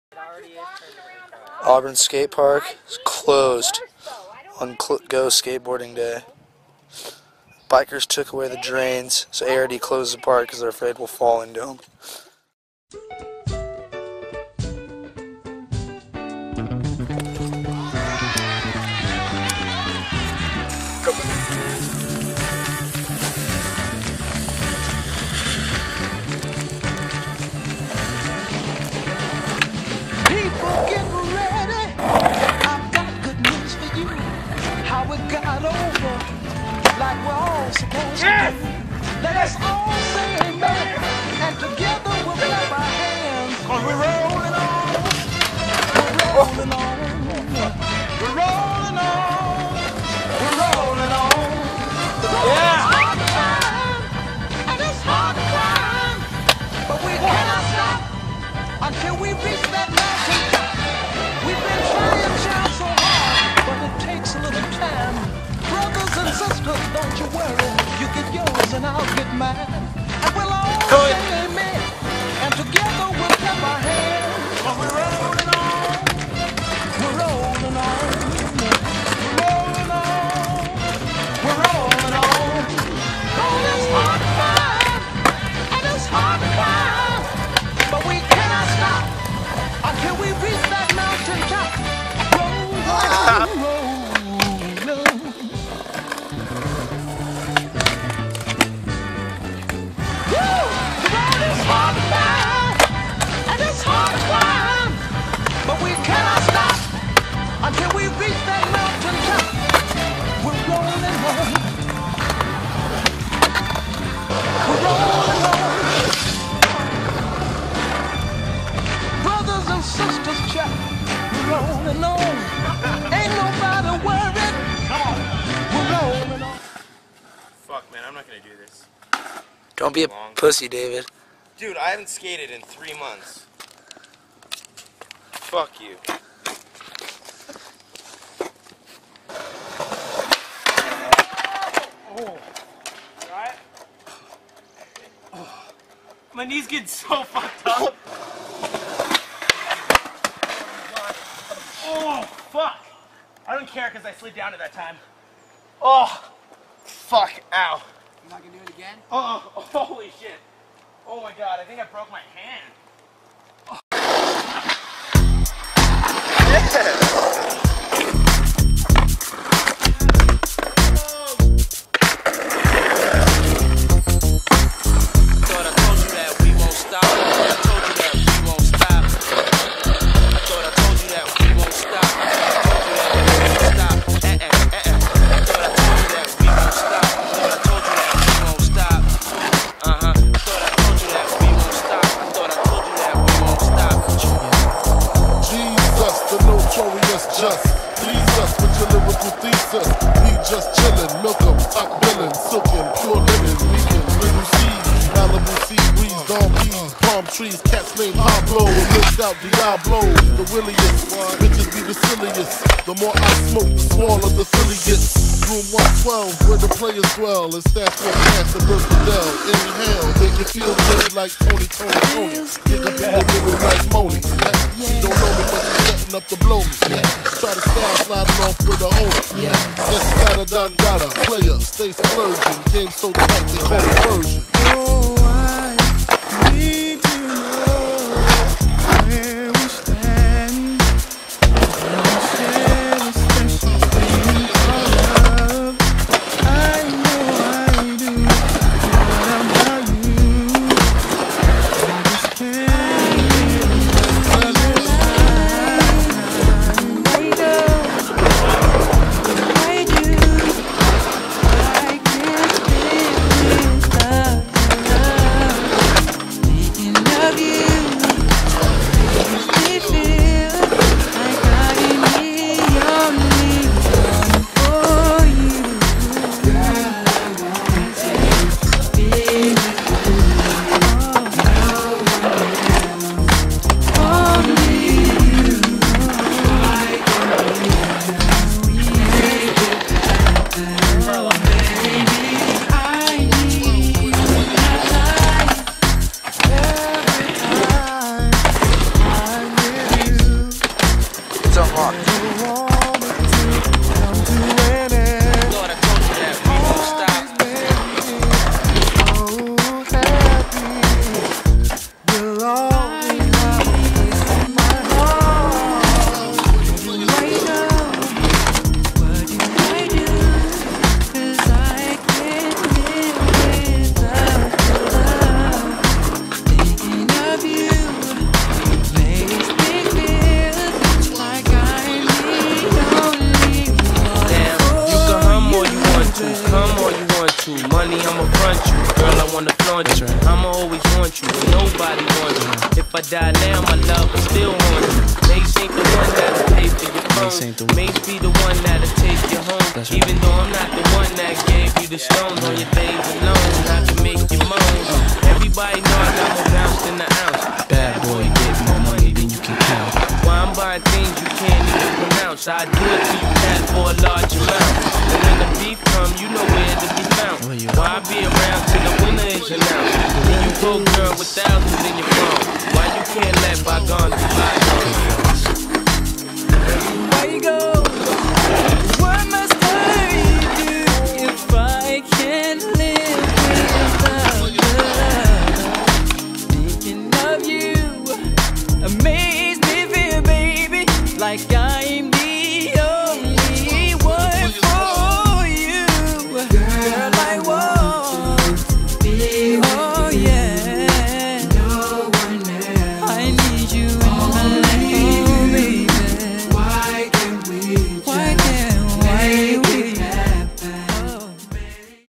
Go Skateboarding Day
Nelson Warren Brandon and David skating on go skate day